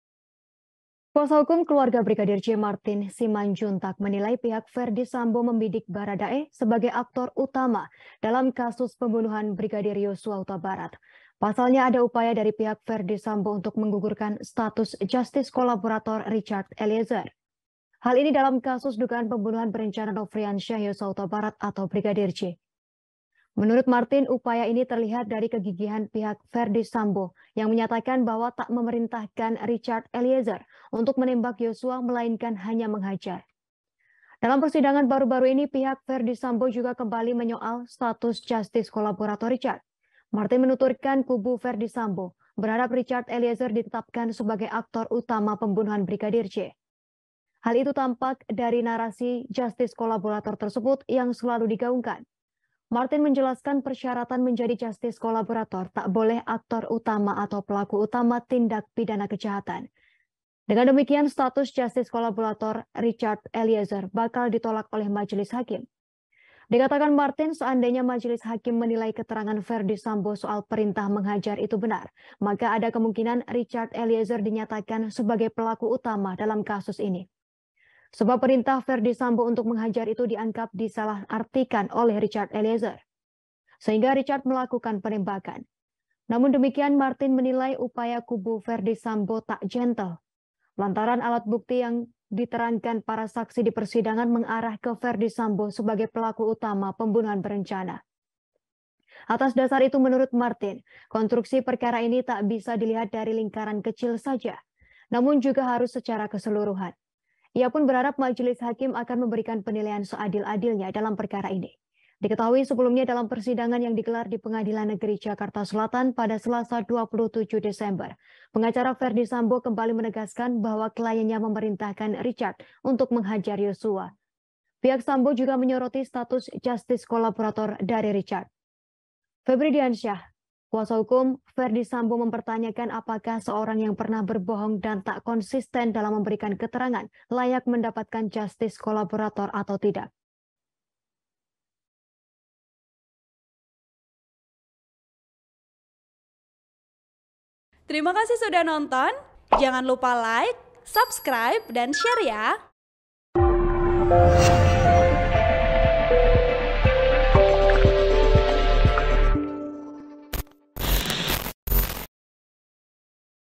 Ferdy Sambo Dinilai Ingin Jadikan Bharada E Aktor Utama Pembunuhan Brigadir J, Persoalkan Status JC
TRIBUN-VIDEO.COM - Kuasa hukum keluarga Brigadir J, Martin Simanjuntak menilai pihak Ferdy Sambo membidik Bharada E sebagai aktor utama dalam kasus pembunuhan Yosua.

Pasalnya, ada upaya dari pihak Ferdy Sambo untuk menggugurkan status justice collaborator (JC) Richard Eliezer.

Hla ini dalam kasus dugaan pembunuhan berencana Nofriansyah Yosua Hutabarat atau Brigadir J.

Menurut Martin, upaya ini terlihat dari kegigihan pihak Sambo menyatakan bahwa tak memerintahkan Richard untuk menembak Yosua, melainkan hanya menghajar.

Dalam persidangan baru-baru ini, pihak Sambo juga kembali menyoal status justice collaborator Richard.

Martin menuturkan, kubu Sambo berharap Richard ditetapkan sebagai aktor utama pembunuhan Brigadir J.

Hal itu nampak dari narasi JC tersebut yang selalu digaungkan.

Martin menjelaskan persyaratan menjadi justice collaborator tidak boleh aktor utama atau pelaku utama tindak pidana kejahatan.

Dengan demikian, status JC Richard bakal ditolak oleh Majelis Hakim.

Dikatakan Martin, seandainya Majelis Hakim menilai keterangan Sambo soal perintah menghajar itu benar, maka, ada kemungkinan Richard dinyatakan sebagai pelaku utama dalam kasus ini.

Sebab, perintah Sambo untuk menghajar itu dianggap  disalahartikan oleh Richard.

Sehingga Richard melakukan penembakan.

Namun demikian, Martin menilai, upaya kubu Sambo ini tidak gentle.

Lantaran, alat bukti dan keterangan para saksi di persidangan mengarah ke Ferdy Sambo sebagai pelaku utama pembunuhan berencana.

Atas dasar itu, menurut Martin, kontruksi perkara ini tidak bisa dilihat dari lingkaran kecil saja.

Namun juga harus secara keseluruhan.

Ia pun berharap Majelis Hakim akan memberikan penilaian seadil-adilnya dalam perkara ini.

Diketahui sebelumnya, dalam persidangan yang digelar di Pengadilan negeri Jakarta Selatan pada Selasa (27/12/2022), pengacara Sambo kembali menegaskan bahwa kliennya memerintahkan Richard untuk menghajar Yosua.

Pihak Sambo juga menyoroti status justice collaborator dari Richard.

Febri Diansyah, kuasa hukum Sambo mempertanyakan apakah seseorang yang pernah berbohong dan tidak konsisten dalam memberikan keterangan layak mendapatkan JC atau tidak.

(Tribun-Video.com/ Kompas.com)

Artikel ini telah tayang di Kompas.com dengan judul "Persoalkan JC, Ferdy Sambo Dinilai Ingin Tarik Bharada E Jadi Aktor Utama Pembunuhan Brigadir J", Klik untuk baca

Editor : Fitria Chusna Farisa

Host: Yustina Kartika
VP:  Yogi Putra Anggitatama